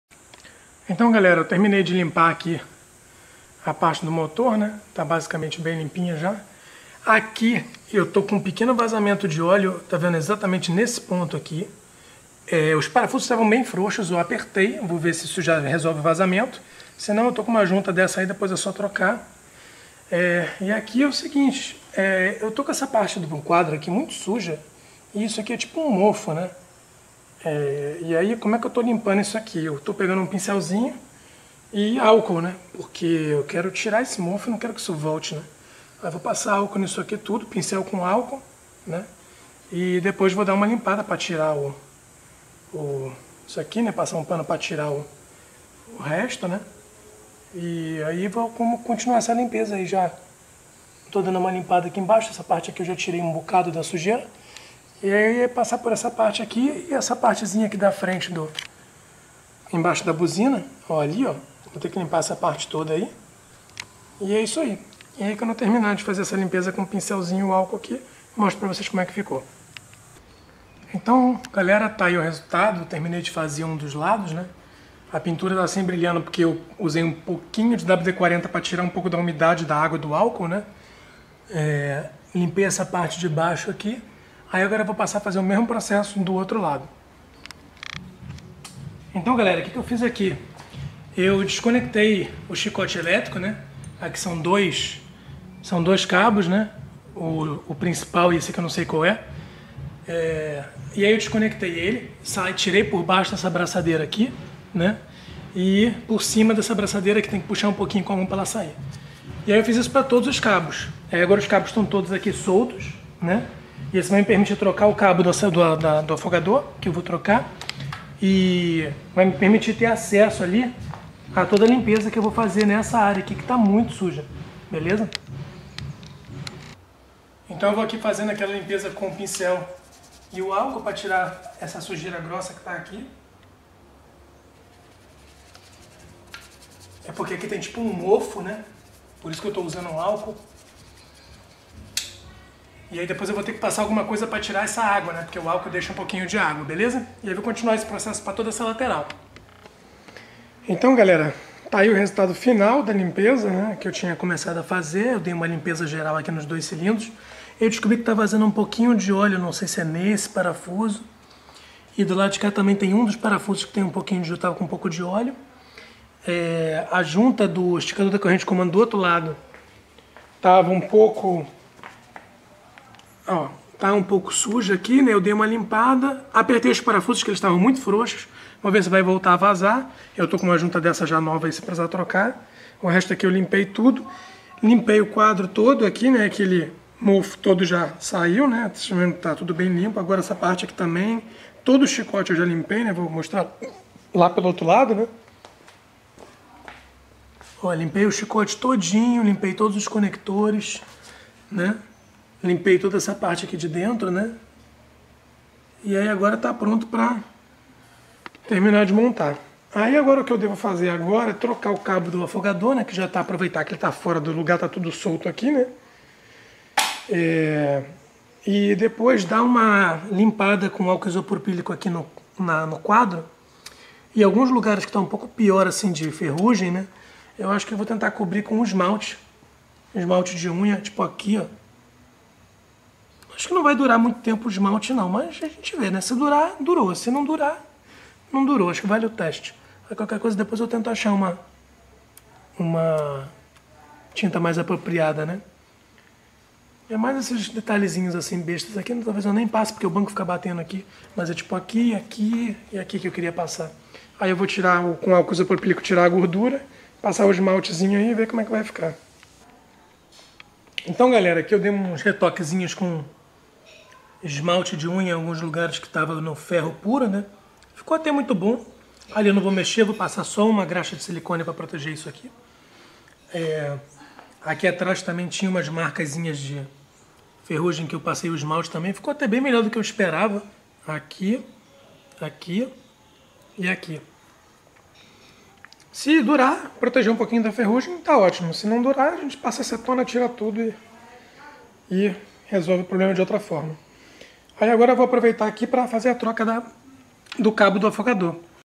XV 250 Virago - Limpeza do quadro
Limpando uma especie de mofo que atacou todo o quadro e o chicote da moto. Pra quem viu o video do filtro de ar, era bem parecido.